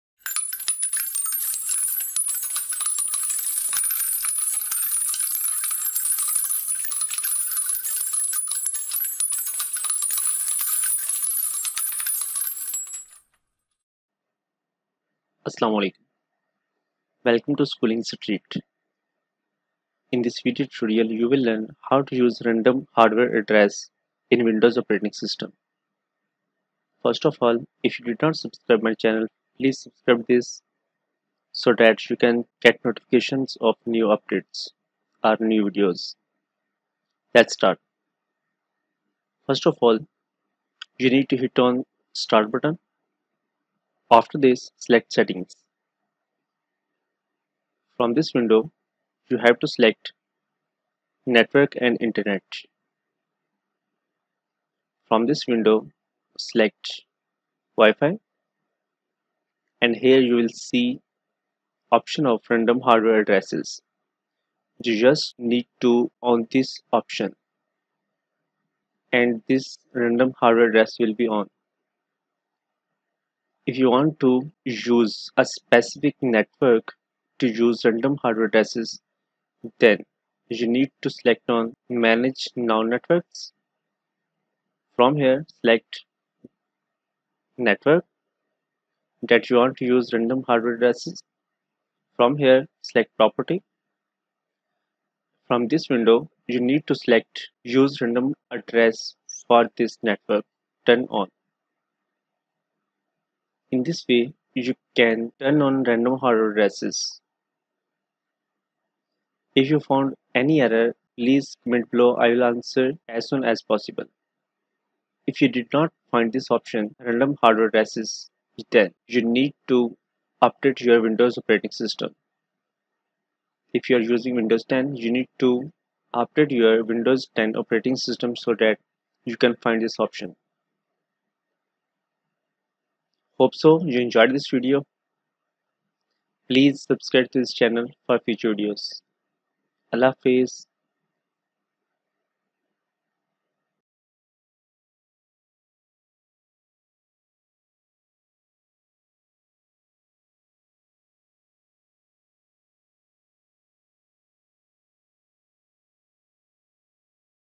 How To Use Random Hardware Address in Windows OS
How to use random hardware addresses
Select the Start button, then select Settings } Network & Internet } Wi-Fi } Manage known networks.
Choose a network, then select Properties and choose the setting you want under Use random hardware addresses for this network.